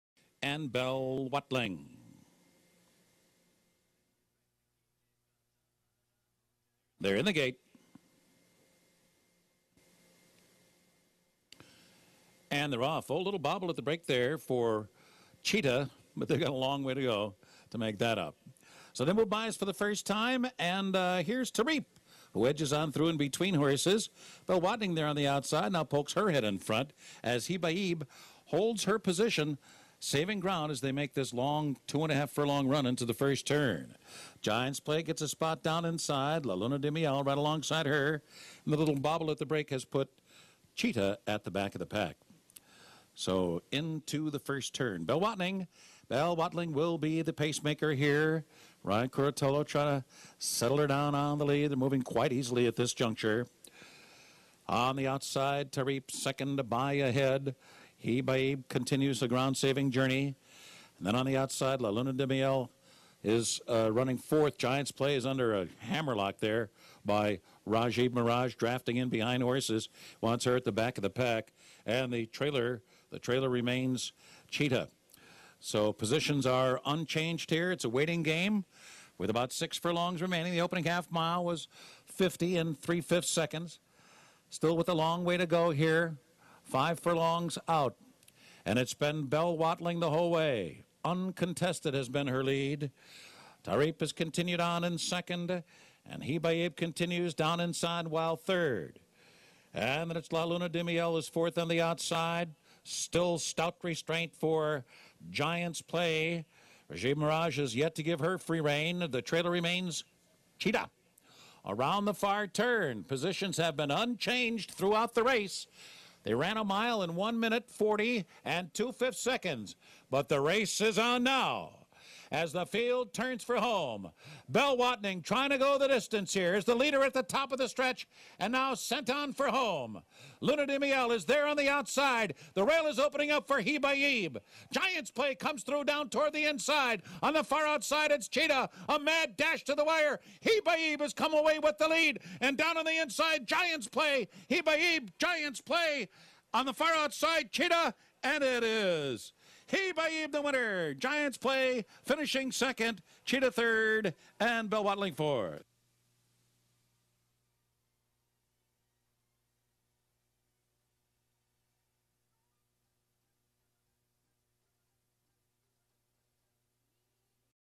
2011 Sheepshead Bay Stakes
Hibaayeb wins the Sheepshead Bay Stakes at Belmont Park on May 28.